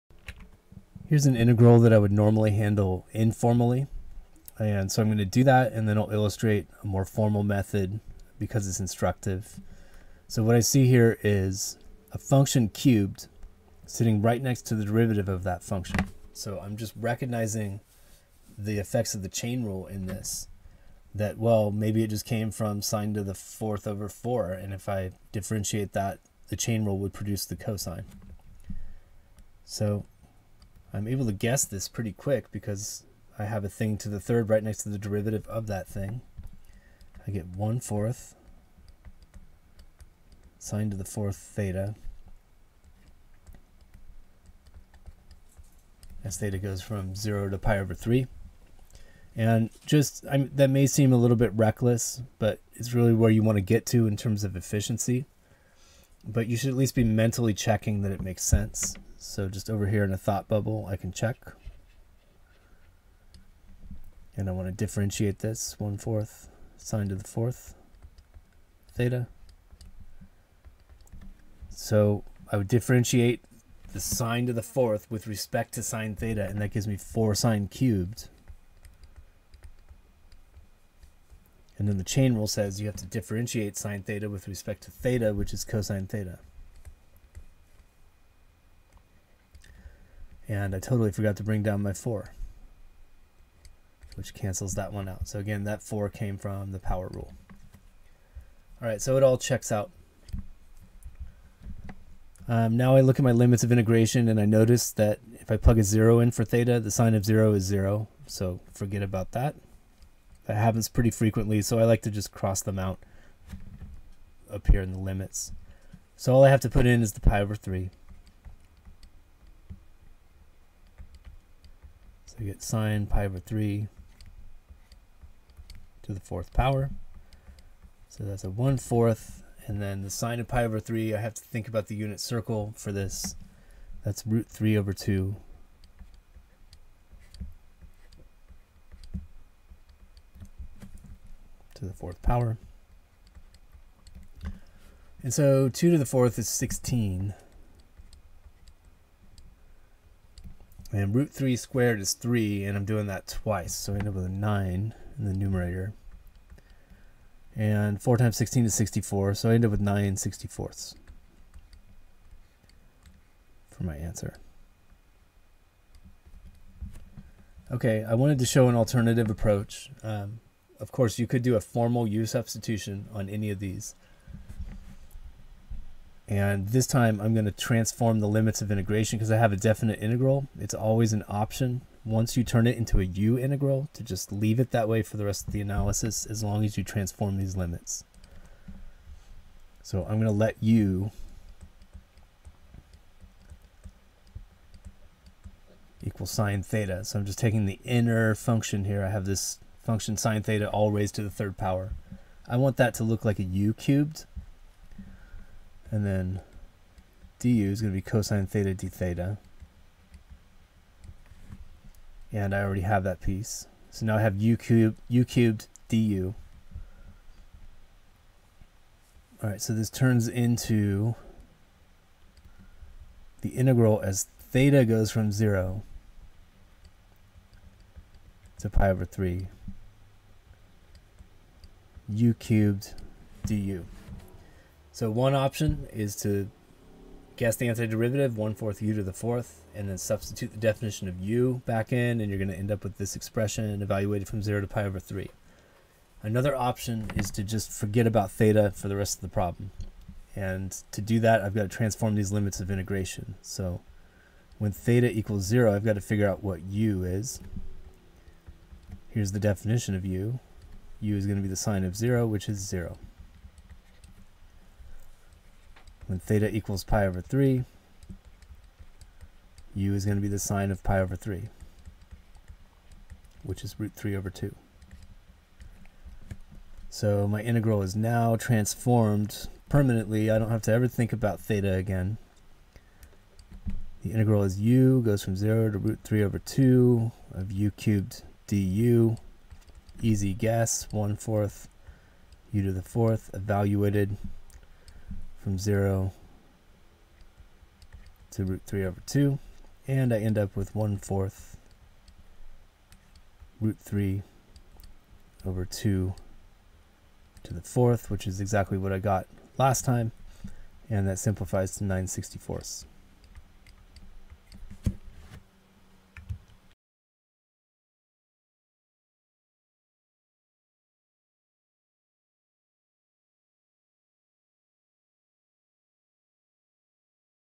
Integral of (sinx)^3*cosx using substitution.
We integrate (sinx)^3*cosx using substitution.  Approaching the integral informally, we can also just recognize that the derivative of sinx is sitting next to a function of sinx, making the "chain rule backwards" easy to see.  See if you can get it without a formal substitution!

Questions or requests?  Post your comments below, and I will usually respond within 24 hours.